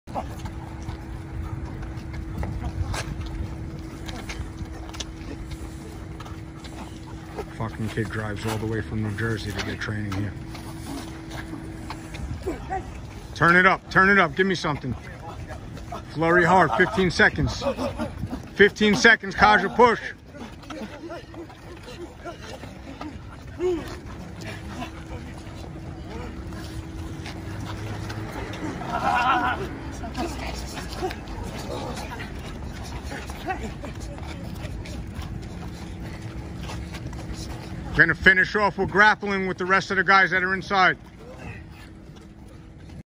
Finish Strong Brooklyn MMA
After an intense workout inside Brooklyn Mixed Martial Arts we took half the squad (those with sneakers) to hit some sprints and SHADOWBOXING boxing outside

BROOKLYN MIXED MARTIAL ARTS 👊